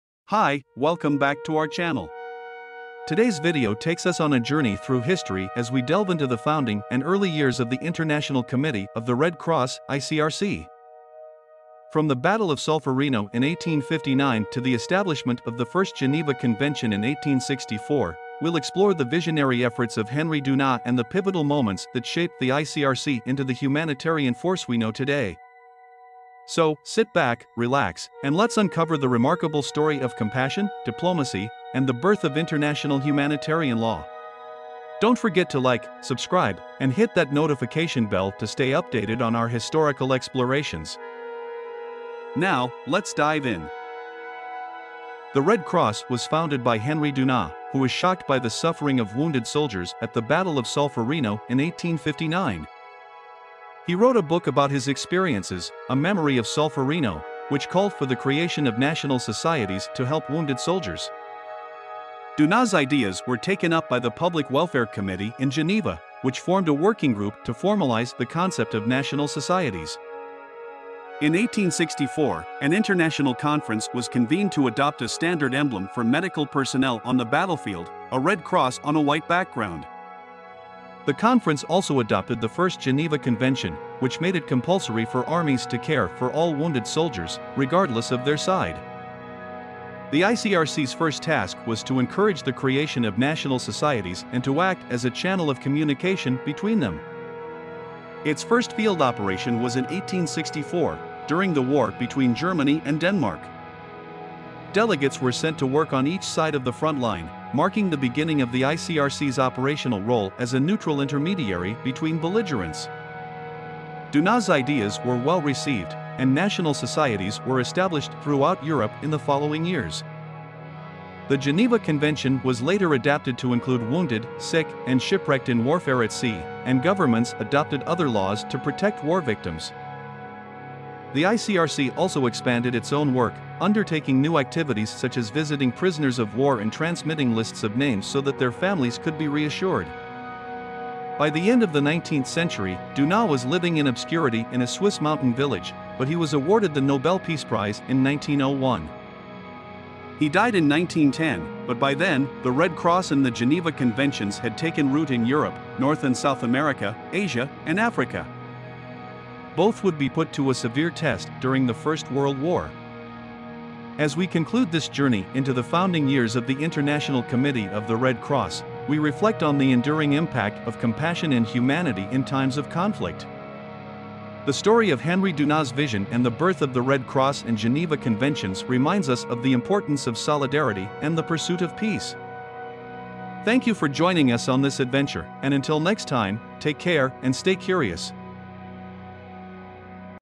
Humanity in Action: The Birth of the Red Cross and Geneva Conventions
On the Frontlines: The Operational Role of the ICRC - Witness the early field missions that marked the ICRC's emergence as a neutral intermediary between warring parties

Henry Dunant: The Man Behind the Vision - Dive deep into the life of the visionary who sparked the creation of the Red Cross and laid the foundation for modern international humanitarian law.

1864 Geneva Convention: The Turning Point - Explore the legal framework that made it compulsory for armies to care for wounded soldiers, setting the stage for humanitarian law.